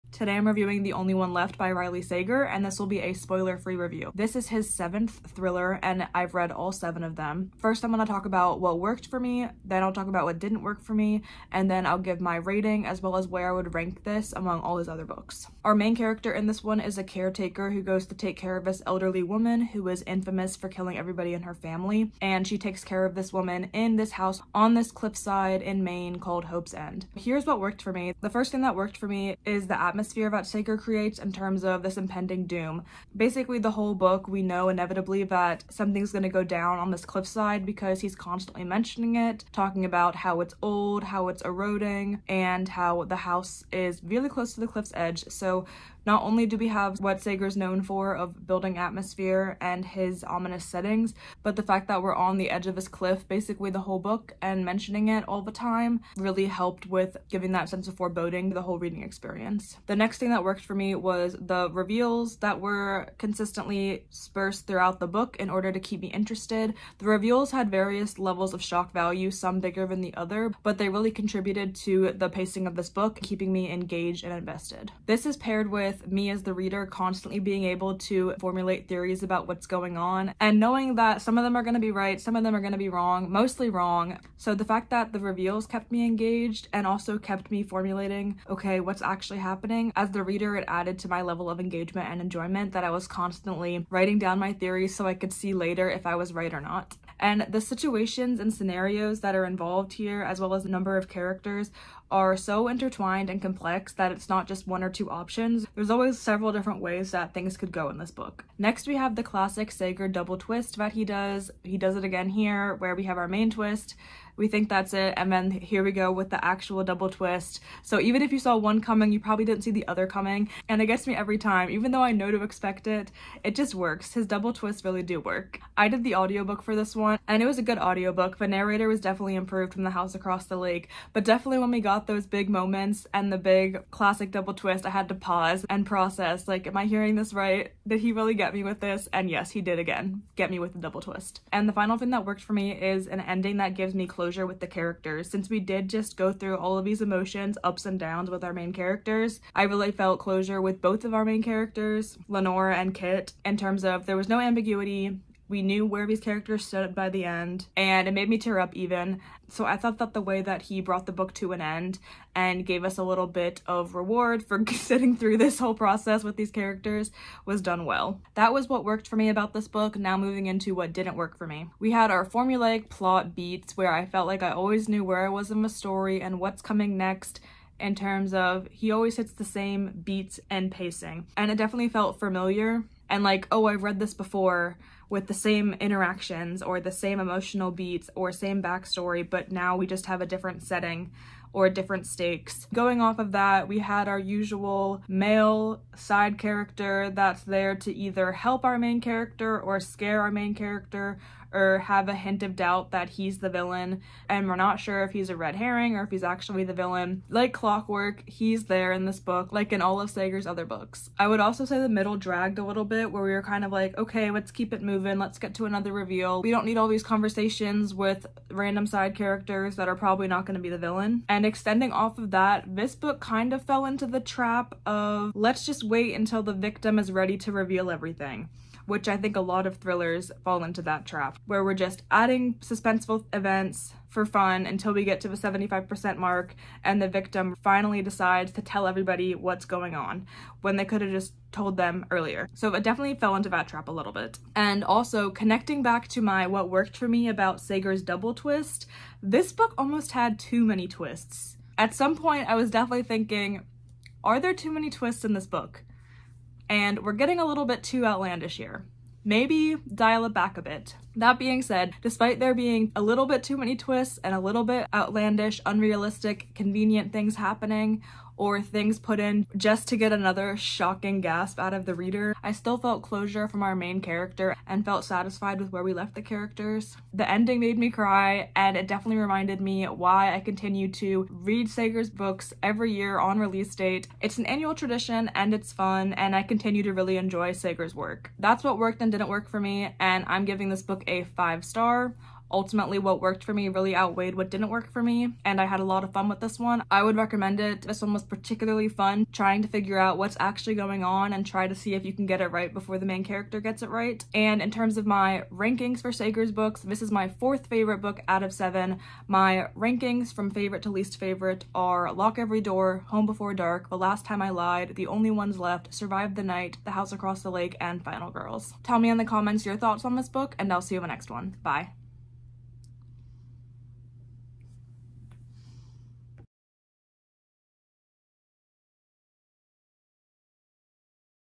The Only One Left by Riley Sager Review | What Worked & What Didn't
Spoiler-free review of The Only One Left by Riley Sager.

0:27 What Worked
2:49 What Didn't
4:50 My Rating
5:04 Updated Riley Sager book rankings